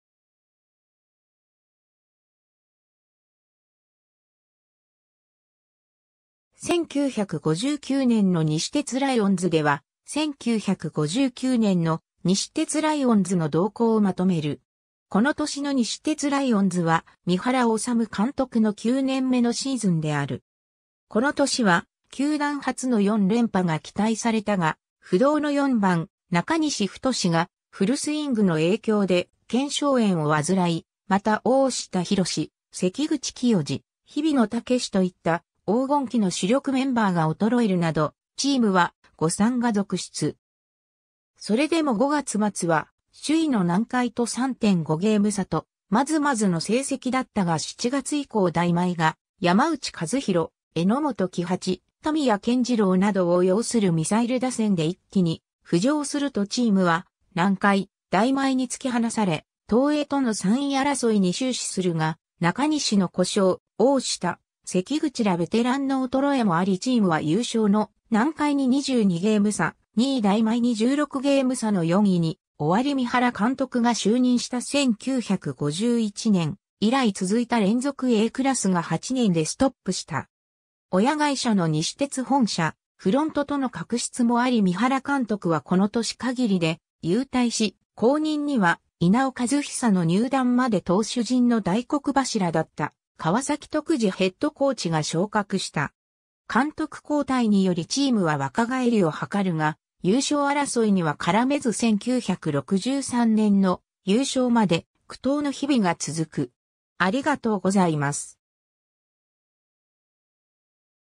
1959年の西鉄ライオンズ
1959年の西鉄ライオンズでは、1959年の西鉄ライオンズの動向をまとめる。
 この年の西鉄ライオンズは、三原脩監督の9年目のシーズンである。
 この年は球団初の4連覇が期待されたが、不動の4番中西太がフルスイングの影響で腱鞘炎を患い、また大下弘・関口清治・日比野武といった黄金期の主力メンバーが衰えるなど、チームは誤算が続出。
それでも5月末は首位の南海と3.5ゲーム差と、まずまずの成績だったが7月以降大毎が山内一弘・榎本喜八・田宮謙次郎（この年阪神から移籍）などを擁するミサイル打線で一気に浮上するとチームは南海・大毎に突き放され、東映との3位争いに終始するが、中西の故障、大下・関口らベテランの衰えもありチームは優勝の南海に22ゲーム差、2位大毎に16ゲーム差の4位に終わり三原監督が就任した1951年以来続いた連続Aクラスが8年でストップした。
親会社の西鉄本社、フロントとの確執もあり三原監督はこの年限りで勇退し、後任には稲尾和久の入団まで投手陣の大黒柱だった川崎徳次ヘッドコーチが昇格した。
監督交代によりチームは若返りを図るが、優勝争いには絡めず1963年の優勝まで苦闘の日々が続く。